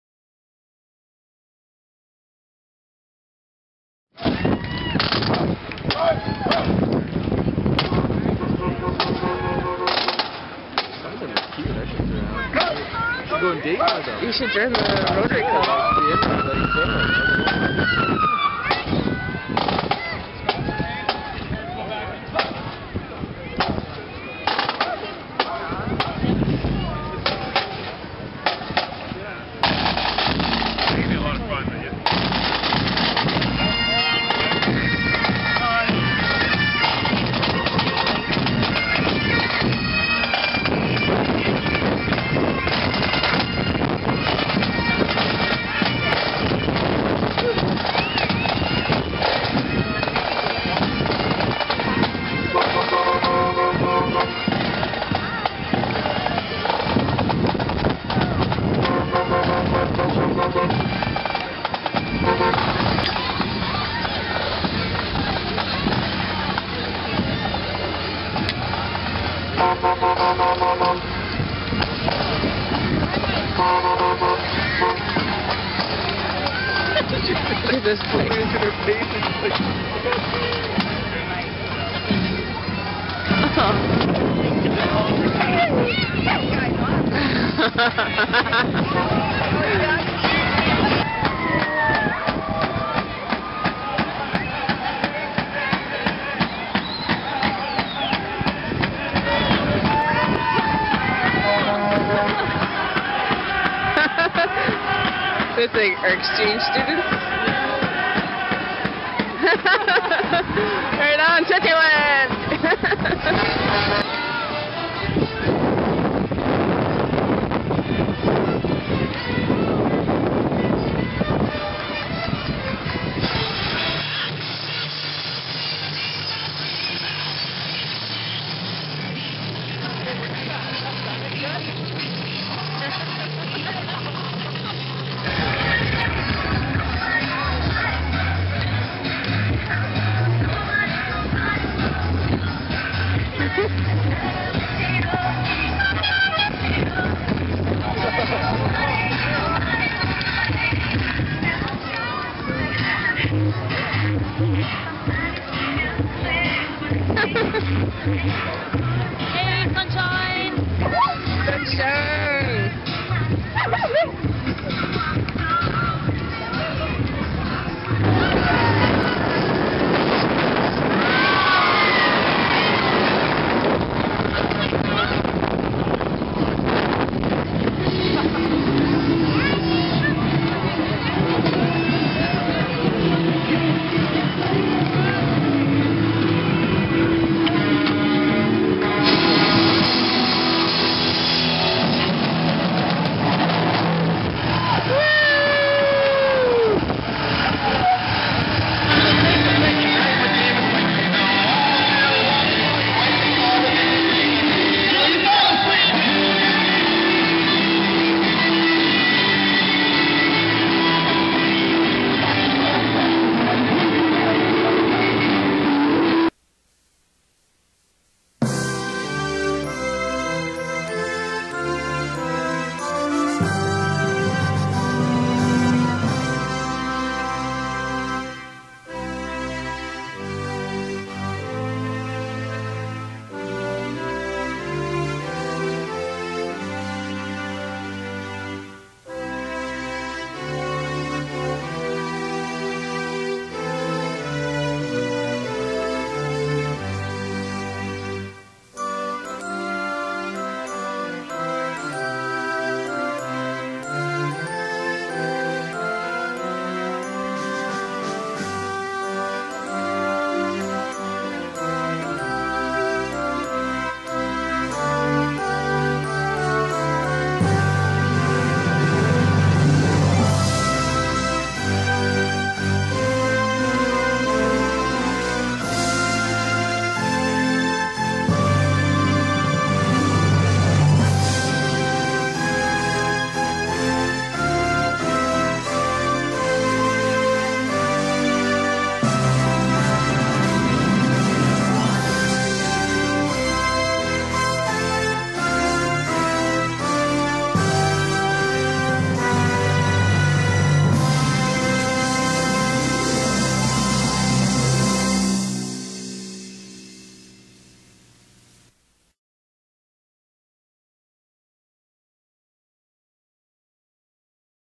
Canada Day 2010
Canada Day Parade in Yellowknife, Northwest Territories